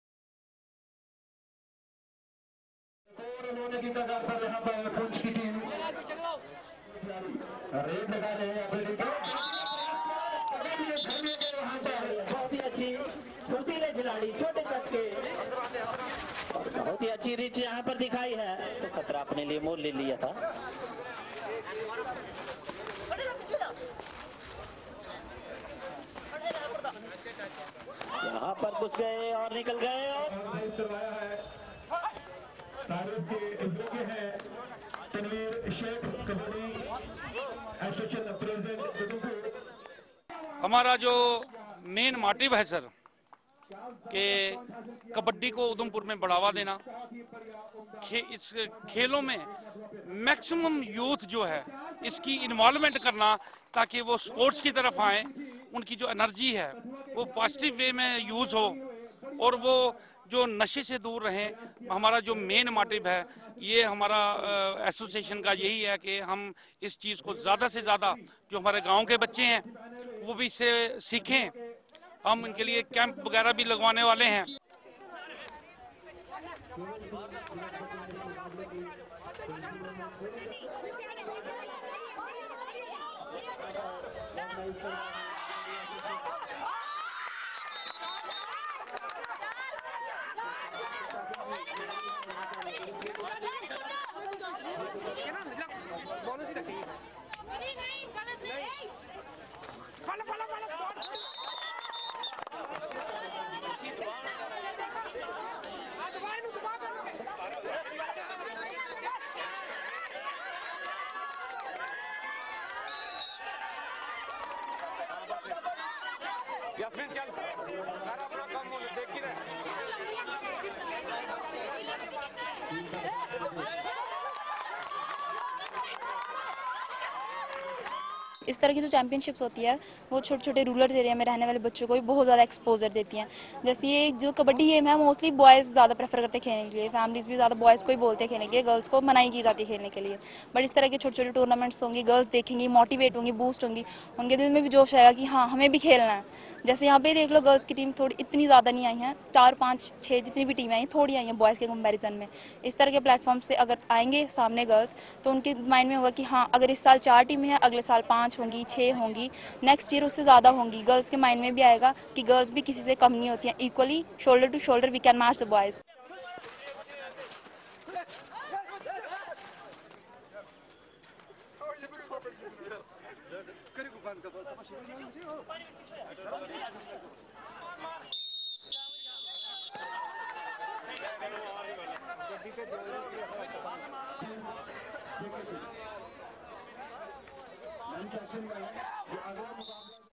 21st state Kabaddi Championship organised in Udhampur
21st state Kabaddi Championship organised in Udhampur